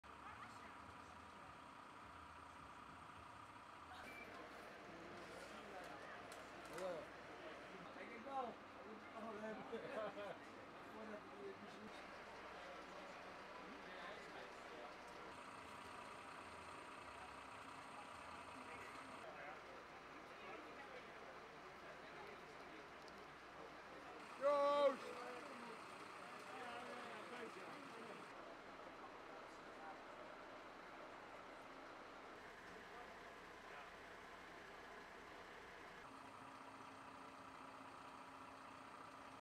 Cafe ontruimdna CO lekkage Meppel
Meppel – Aan de Grote Kerkstraat in Meppel is van zaterdag op zondagnacht Café De Kansel ontruimd. Enkele tientallen bezoekers moesten in allerijl naar buiten in verband met een CO lekkage. De brandweer kwam met spoed ter plaatse en constateerde dat de biertapinstallatie CO lekte. Dit probleem kon ter plaatse worden verholpen waarna het café weer kon worden vrijgegeven. Tijdens de lekkage raakte iemand onwel. Het slachtoffer is in de ambulance na gekeken.